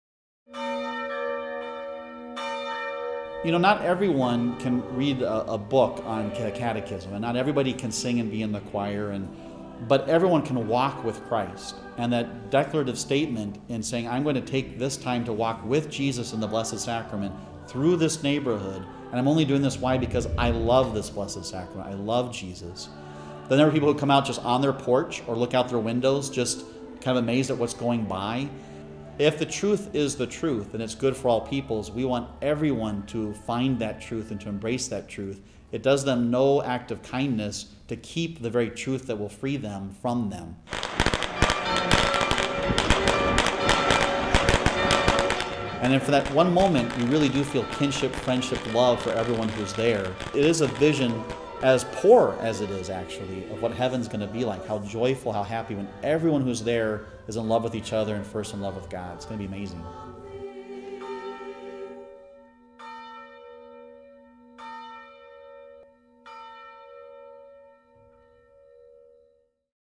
Corpus Christi Procession - A StoryTel Short
Our latest "short", one-minute, video briefly describes Omaha's annual Corpus Christi Procession, as organized by Fr. Damian Cook and many others in the St. Peter and Our Lady of Lourdes parishes of Omaha. This is excerpted from our documentary "St. Peter: Restoring the Sacred in Omaha" but will later be expanded into it's own full documentary about the Real Presence.